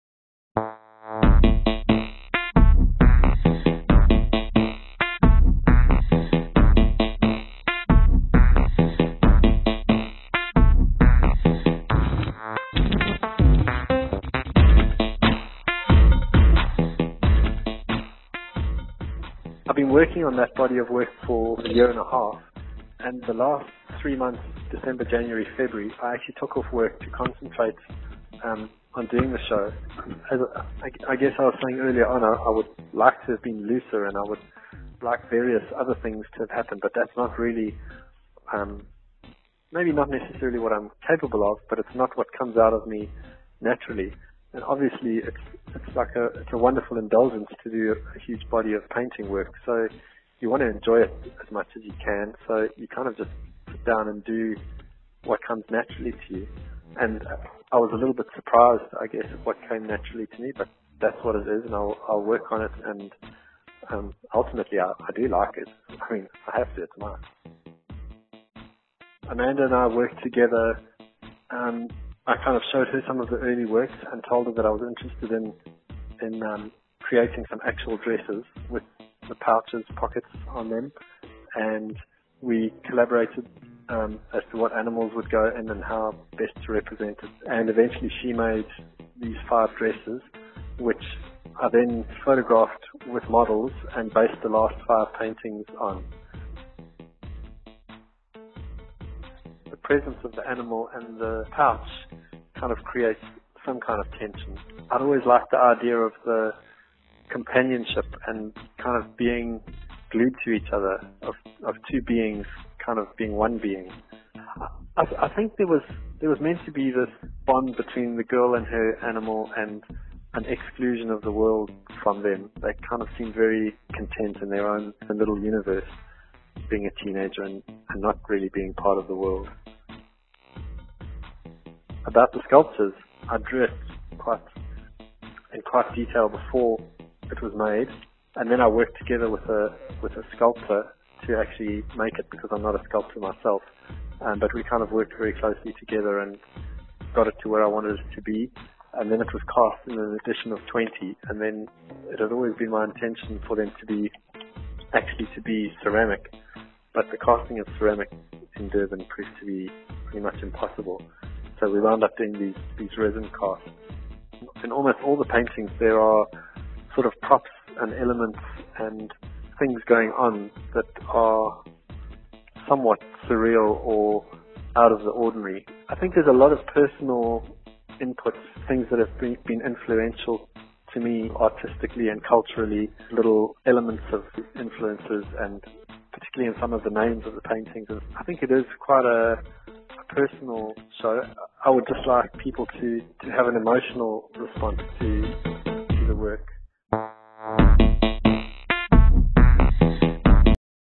Richard Hart on Kind Pockets
Richard Hart, an established Durban-based designer, founded Disturbance Design in 1997, shortly after returning from his studies abroad. Hart sees creating art as a wonderful indulgence to be enjoyed as much as possible, with his focus being on doing what comes naturally. Hart discusses his first art exhibition, Kind Pockets, a series of somewhat surreal portraits of teenage girls carrying their animals in pouches.